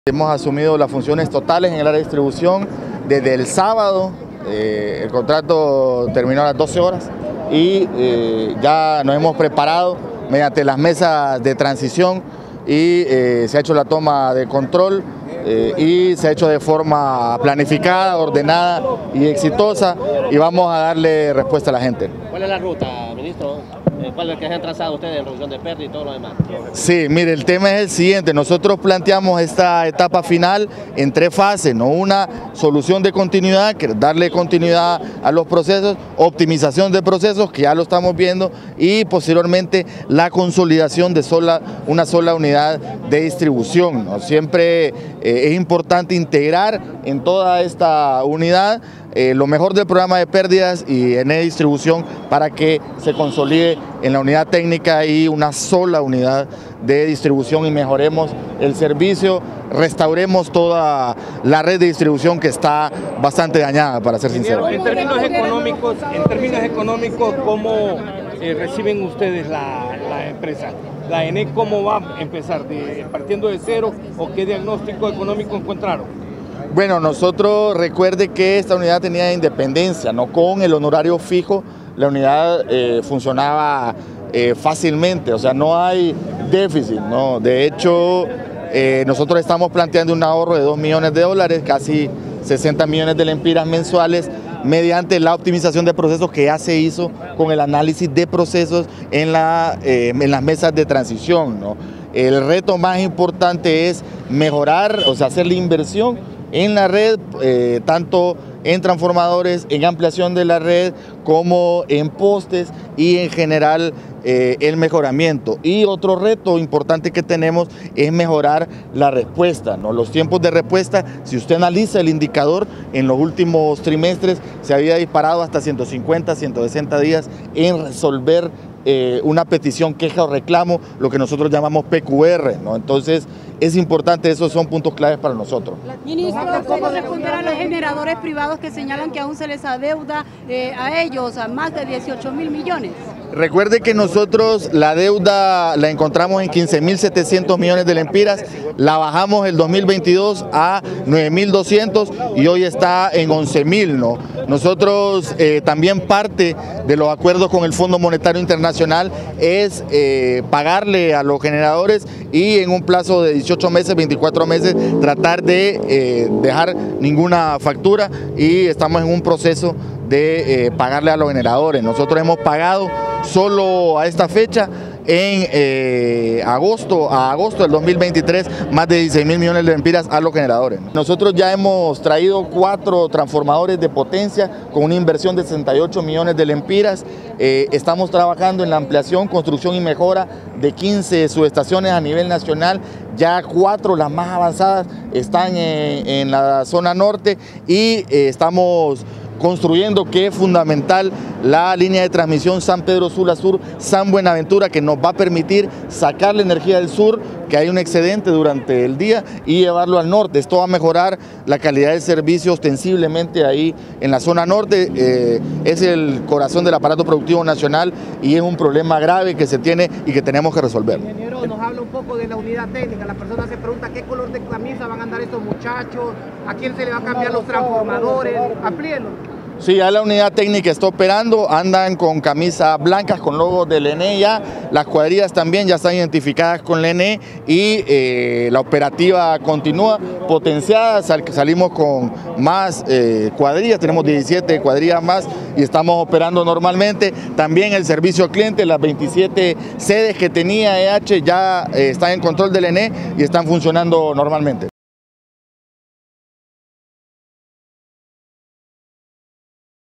Titular de la ENEE explica fases a seguir, posterior asumir la gestión comercial y de facturación
El titular de la Empresa Nacional de Energía Eléctrica (ENEE), Erick Tejada, explicó este lunes cuáles serán las seguir, posterior a que la estatal eléctrica asumió funciones en la gestión comercial, de facturación y base que estaba a cargo de la Empresa Energía Honduras (EEH).